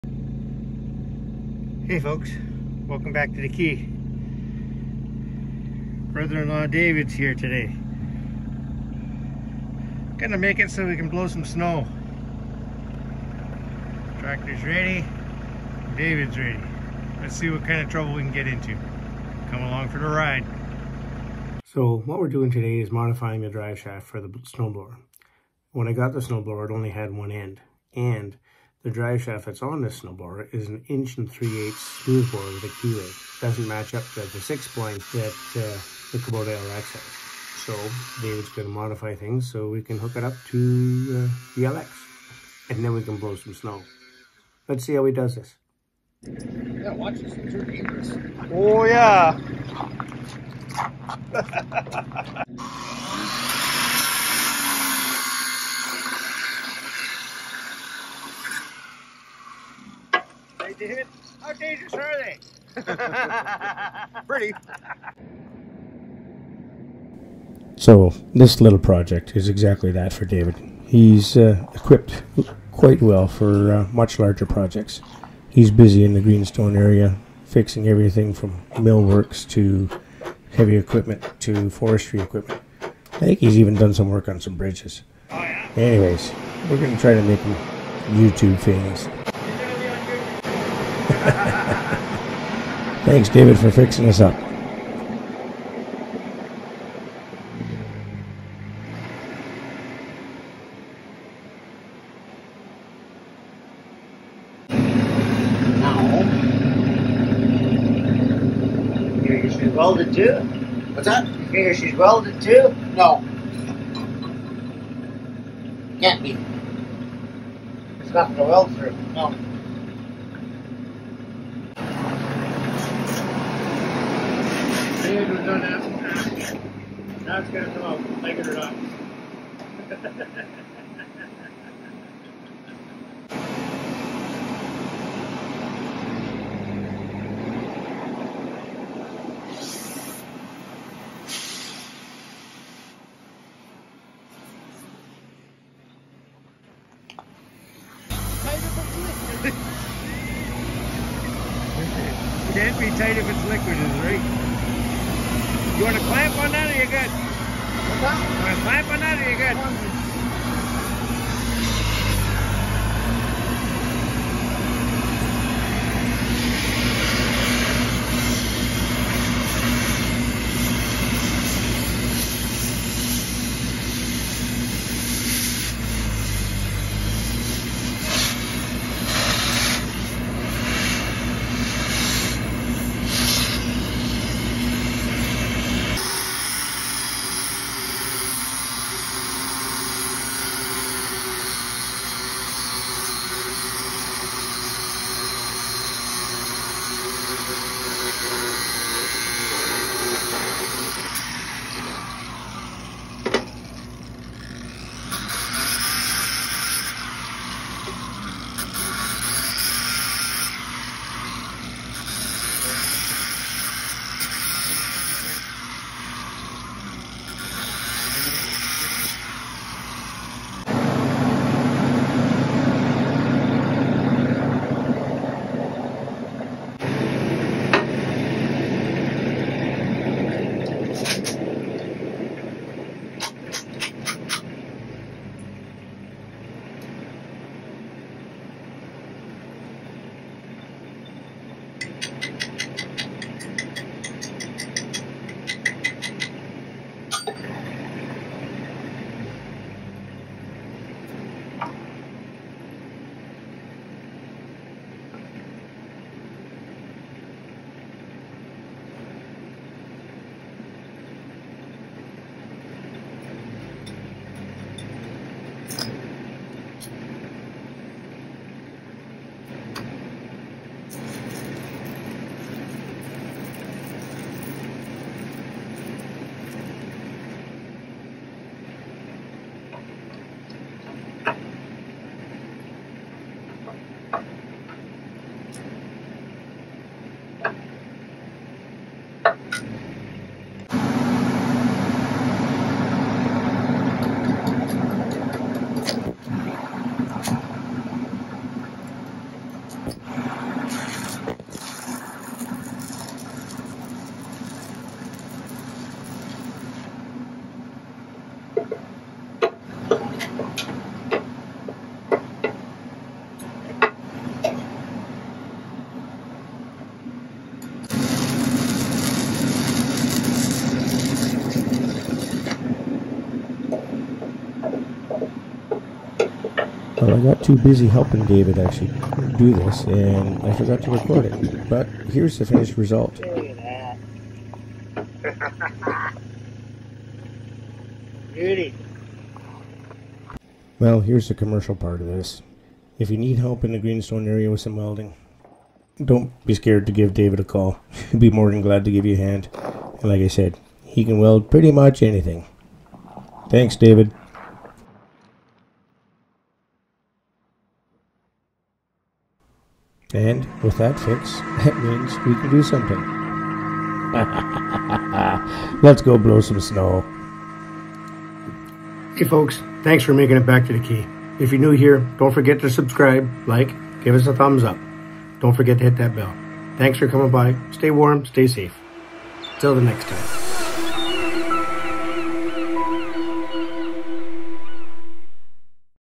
Episode #100 Broken Driveshaft PTO Snowblower
With the temperature today at -20ºC/-4ºF. Brother-in-law David comes to the Cay and gets us ready to blow snow.
A master in his trade David makes quick work of repairing the PTO driveshaft.

Liquid payment made later. 🍻